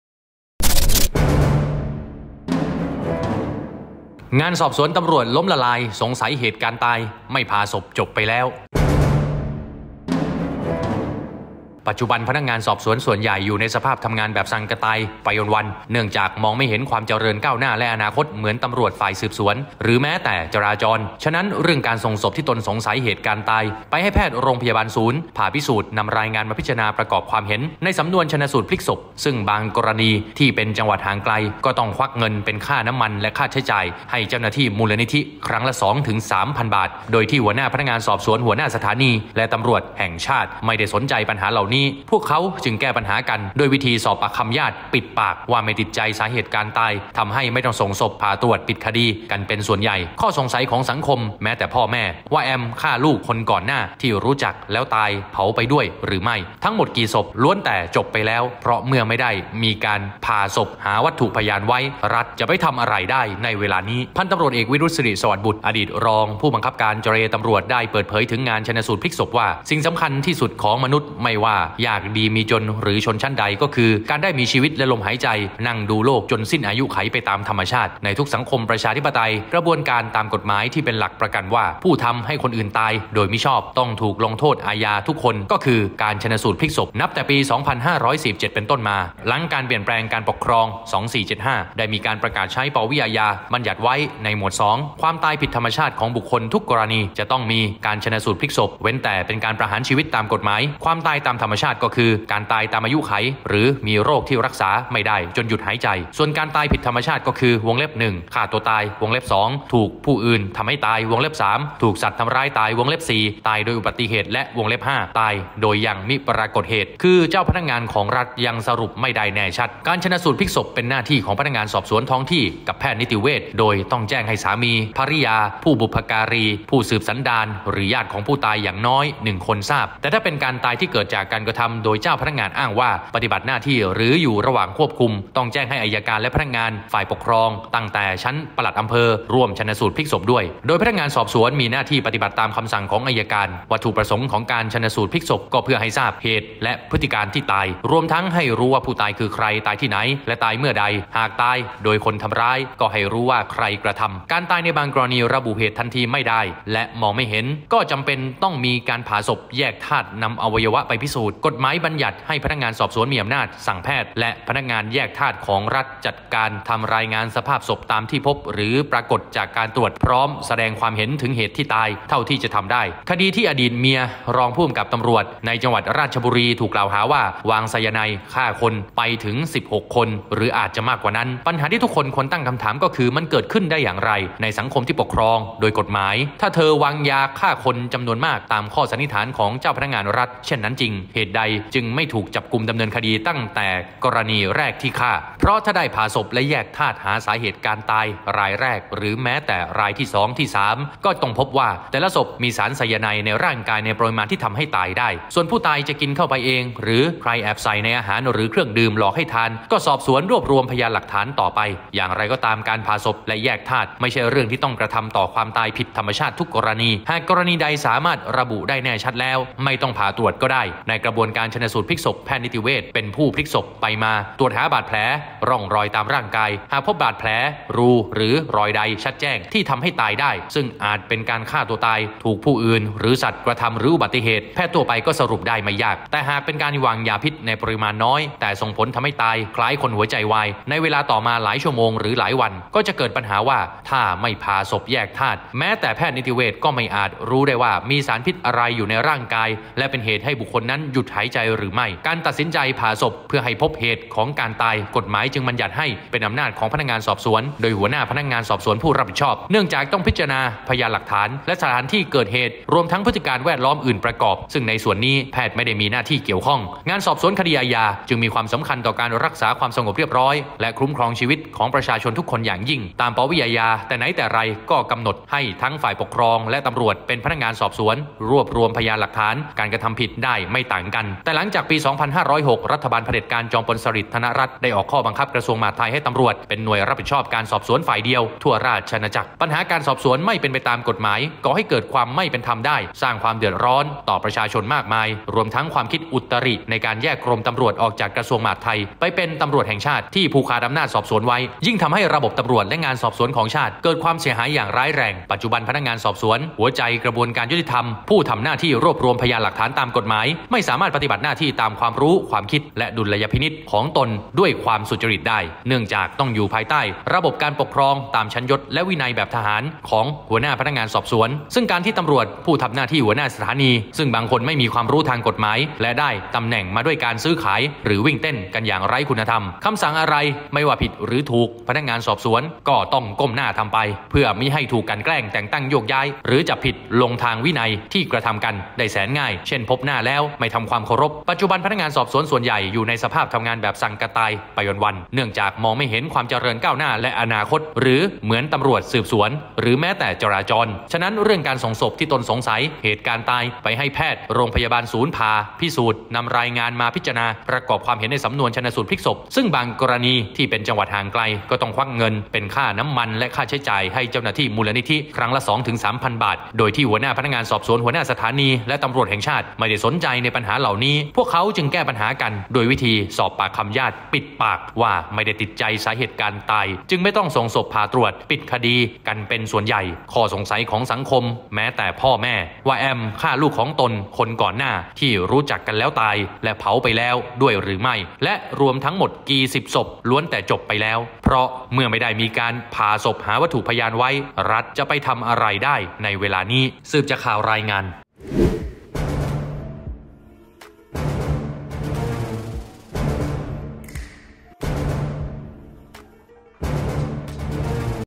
‘งานสอบสวน ตร.’ ล้มละลาย สงสัย ‘เหตุการตาย’ ไม่ผ่าศพ จบไปแล้ว!
โดย : พันตำรวจเอกวิรุตม์ ศิริสวัสดิบุตร

สิ่งสำคัญที่สุดของมนุษย์ไม่ว่ายากดีมีจนหรือชนชั้นใดก็คือ การได้มีชีวิตและลมหายใจนั่งดูโลกจนสิ้นอายุขัยไปตามธรรมชาติ

ในทุกสังคมประชาธิปไตย กระบวนการตามกฎหมายที่เป็นหลักประกันว่าผู้ ทำให้คนอื่นตายโดยมิชอบ ต้องถูกลงโทษอาญาทุกคนก็คือ

“การชันสูตรพลิกศพ”

นับแต่ปี พ.ศ.2477 เป็นต้นมา หลังเปลี่ยนแปลงการปกครอง พ.ศ.2475 ได้มีการประกาศใช้ ป.วิ อาญา บัญญัติไว้ในหมวด 2

ความตายผิดธรรมชาติของบุคคล ทุกกรณีจะต้องมี การชันสูตรพลิกศพ เว้นแต่เป็นการประหารชีวิตตามกฎหมาย ความตายตามธรรมชาติก็คือ การตายตามอายุขัยหรือมีโรคที่รักษาไม่ได้จนหยุดหายใจ

ส่วน ตายผิดธรรมชาติ ก็คือ (1) ฆ่าตัวตาย (2) ถูกผู้อื่นทำให้ตาย (3) ถูกสัตว์ทำร้ายตาย (4) ตายโดยอุบัติเหตุ และ (5) ตายโดยยังมิปรากฏเหตุ

คือ เจ้าพนักงานของรัฐยังสรุปไม่ได้แน่ชัด